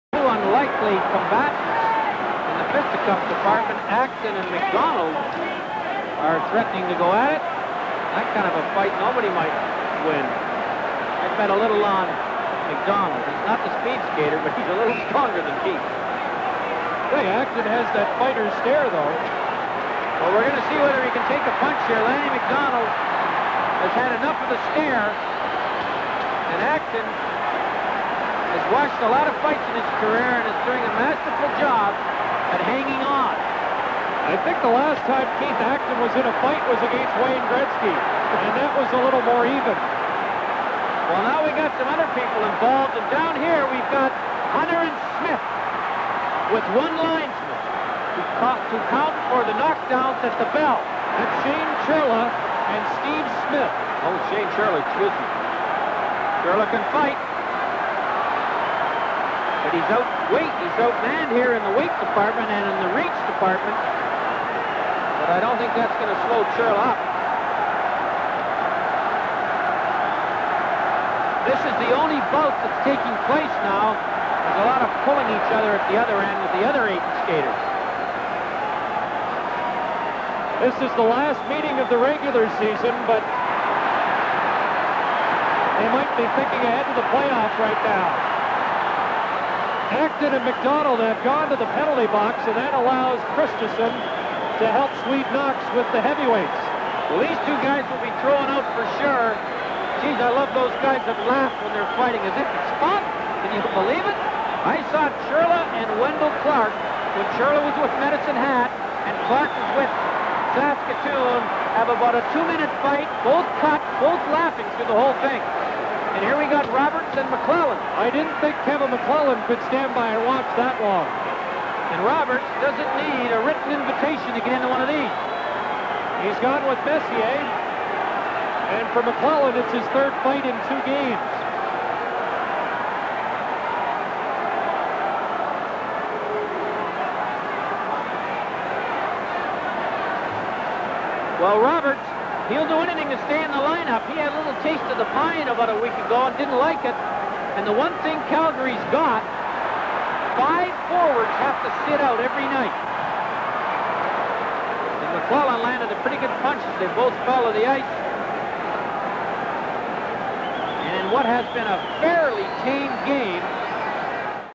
Oilers vs Flames Mar 5, 1988
Kevin McClelland vs Gary Roberts, Steve Smith vs Shane Churla, Keith Acton vs Lanny McDonald from the Edmonton Oilers at Calgary Flames game on Mar 5, 1988.